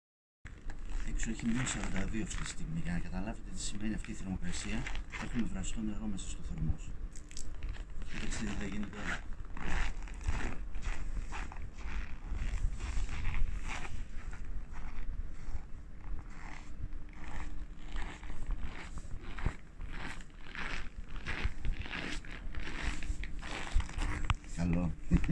-42°C !!
Boiled water experiment at -42°C
Πείραμα με βραστό νερό στους -42°C